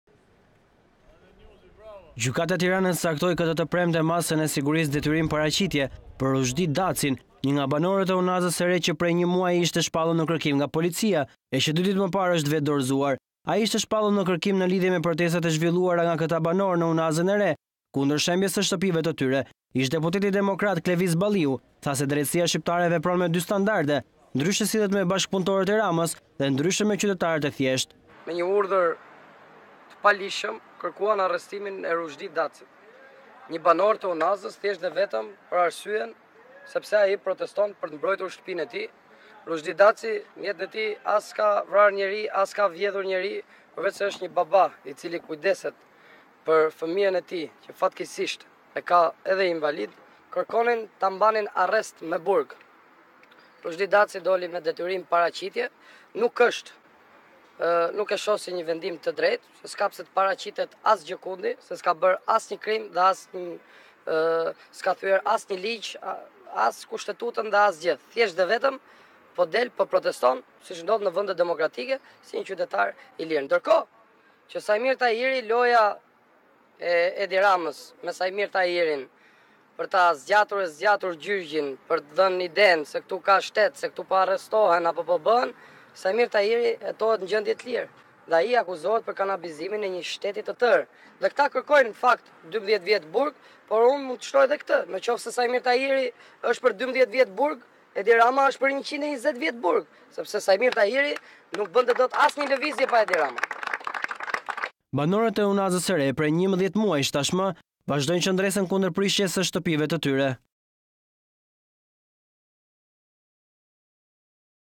Gjykata, dha vendimin për banorin e Unazes, ja cili ishte
Gjykata, detyrim paraqitje per banorin e Unazes
Ruzhdi Daci ne kerkim nga policia prej nje muaji
Daci u vetedorezua ne polici para 2 ditesh
Daci I shpallur ne kerkim per shkak te protestave
Balliu: Daci protestoi kunder shembjes se shtepise
“Nese per tahirin kerkon 12 vjet, per Ramen duhen 120”